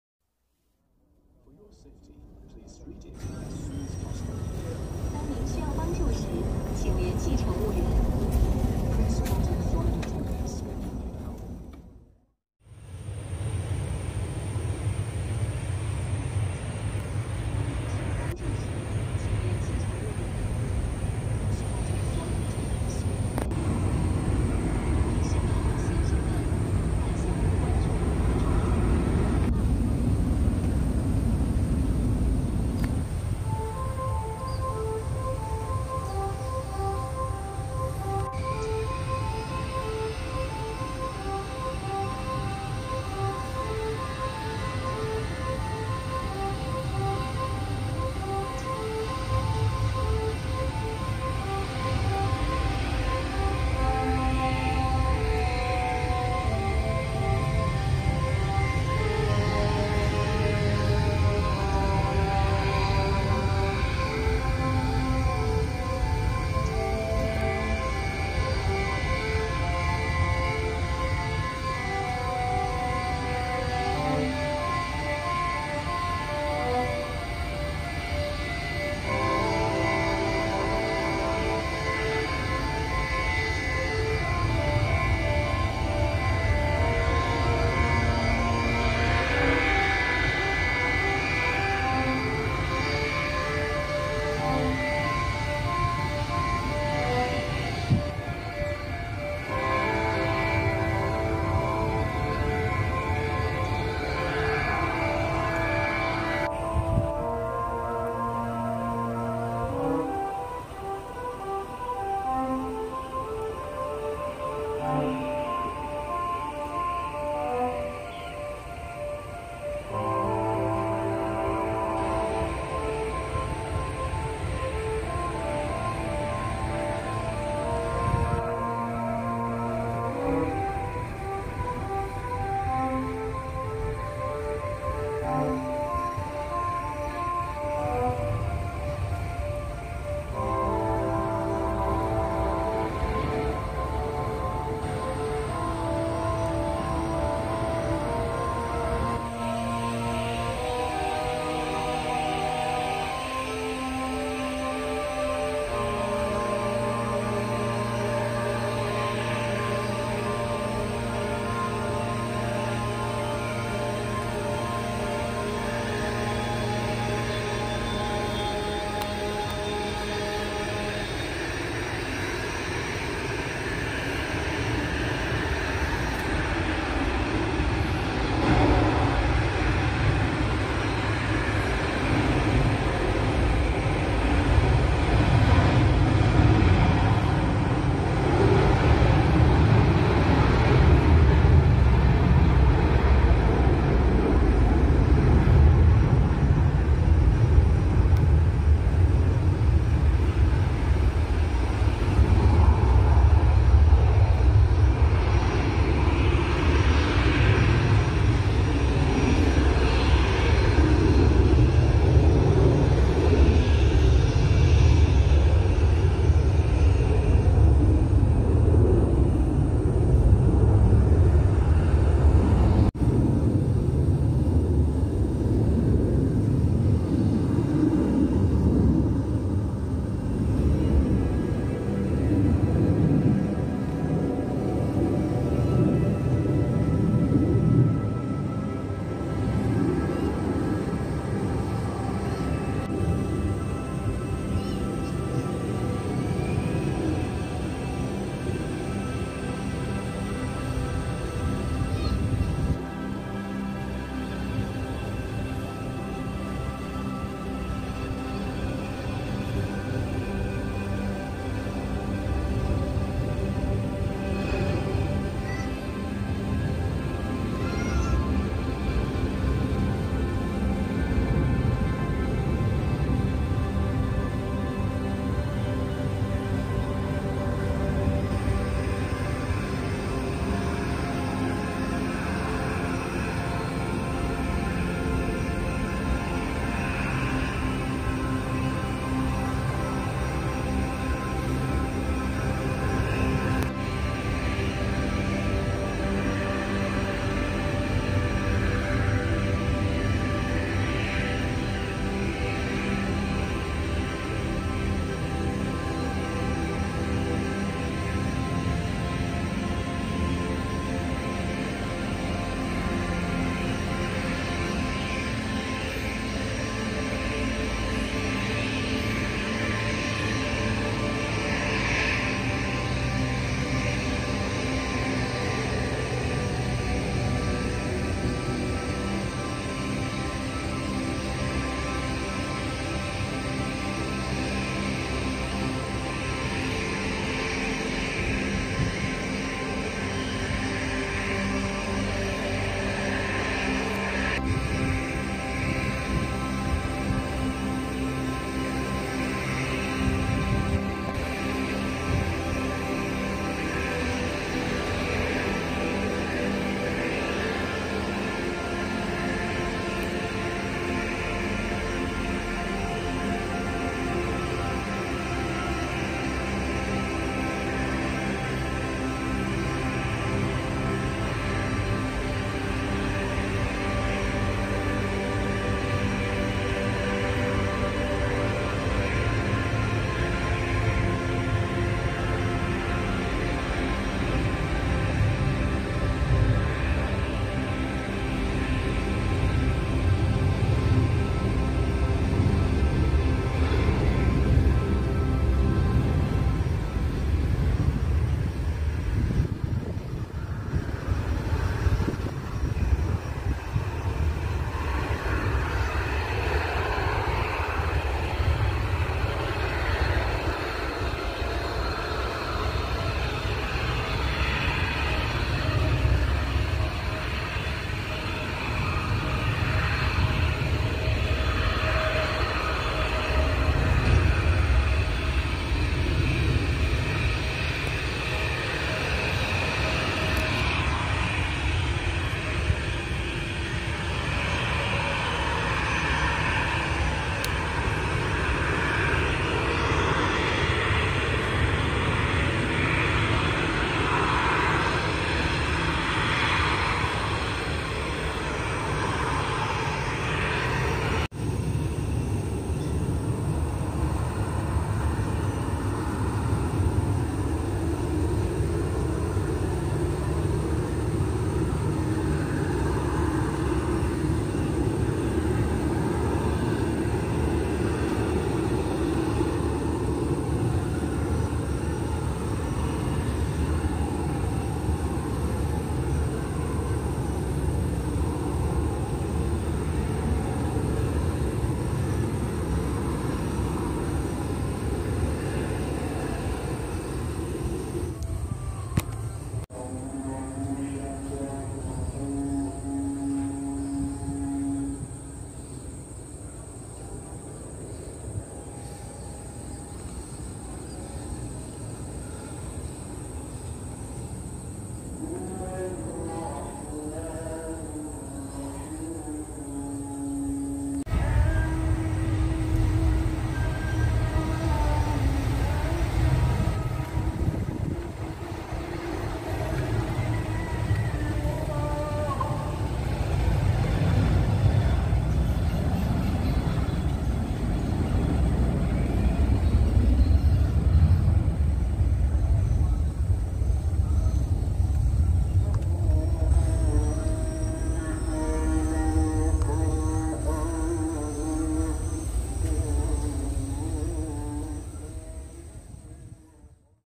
Vlog!Plane Spotting Antrian Pesawat di Makassar Internasional Airport
Video Vlog bandra makassar JR maulana Antrian dan kesibukan Pesawat-Pesawat Terbang Landing dan Take Off dari berbagai Maskapai penerbangan,di Makassar Internasional Airport,south sulawesi,Indonesia